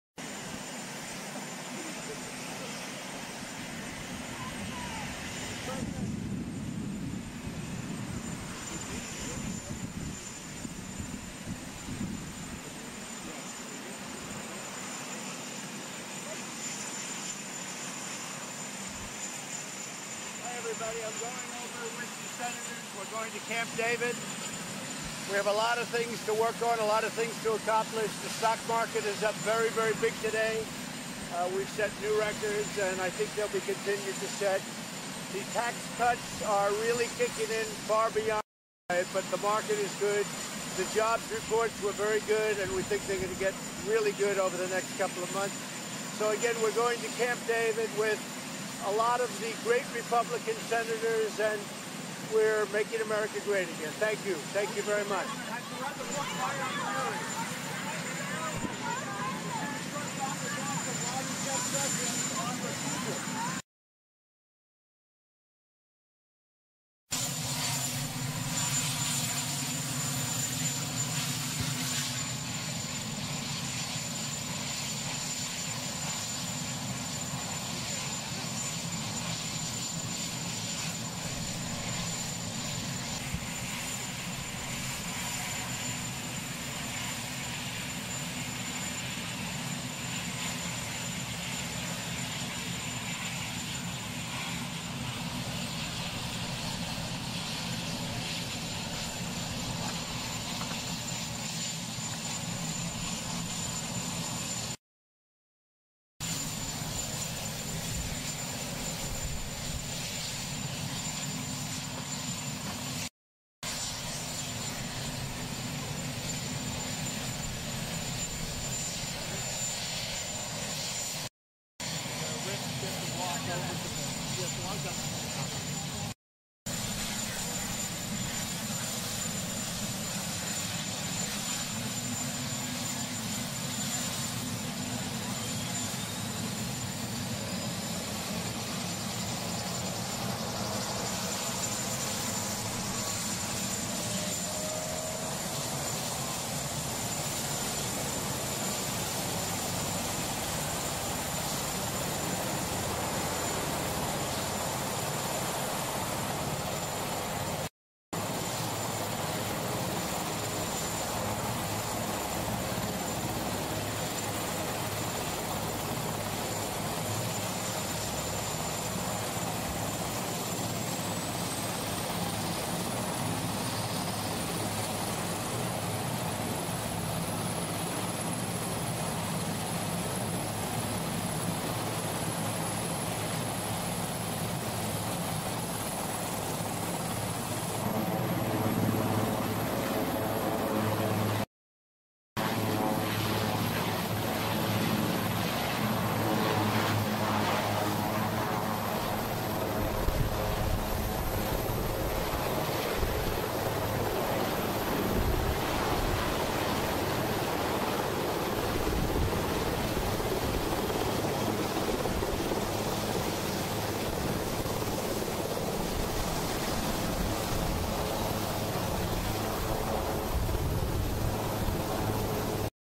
President Trump departs the White House on route to Camp David Jan 5, 2018/ Washington D.C
President Trump departs the White House en route to Camp David. Jan 5, 2018. South Lawn. Washington D.C.